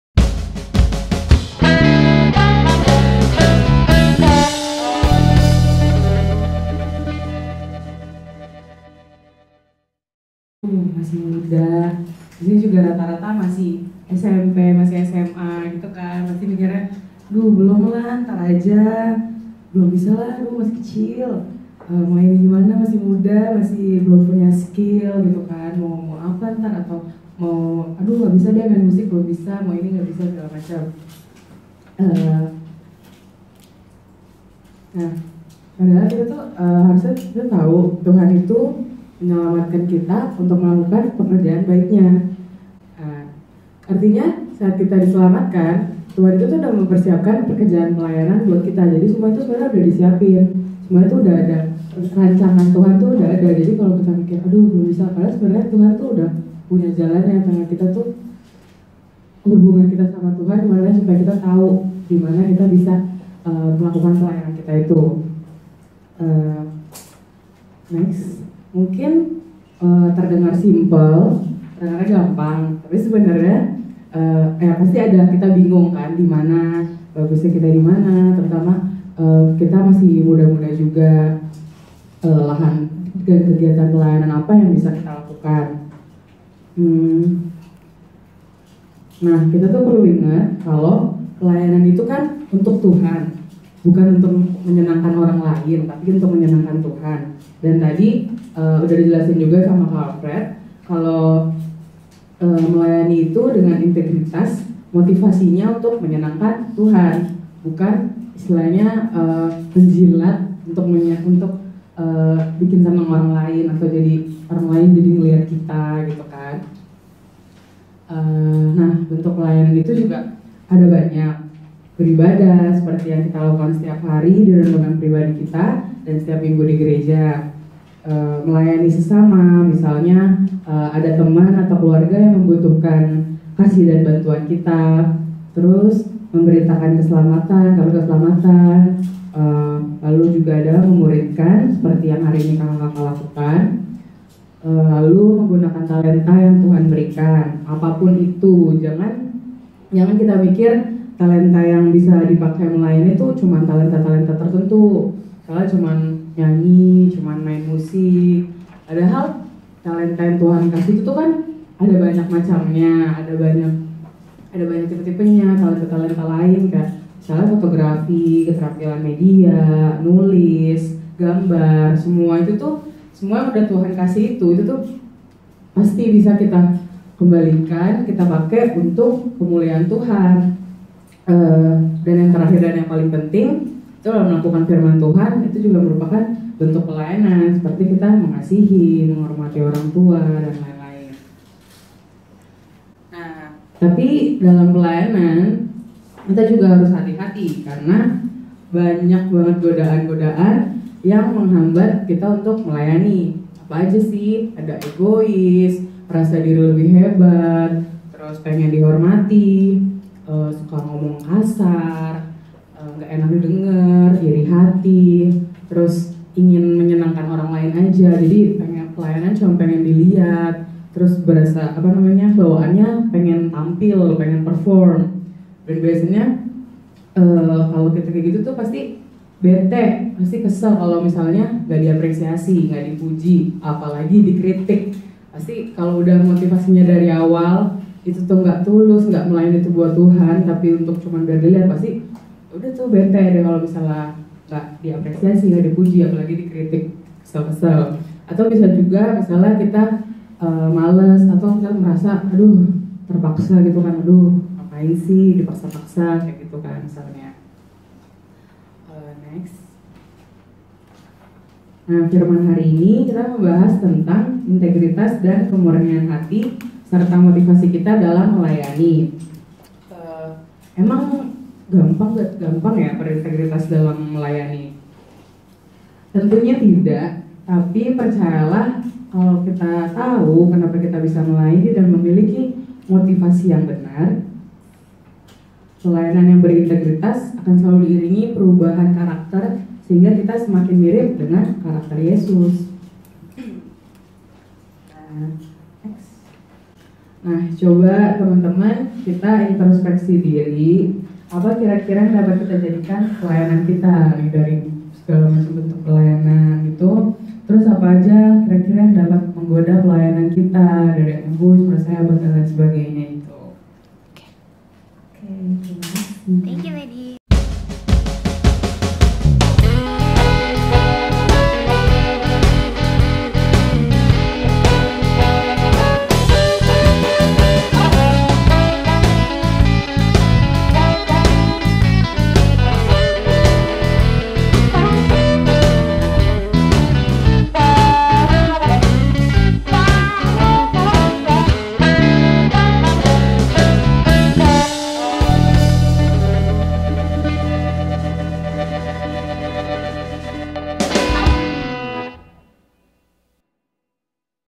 Throw Back PELAYANAN & PENGHAMBAT - Lady Nadira TSD
evaluasi diri kamu suka melayani dimana dan apakah ada penghambat pelayanan dalam hidupmu.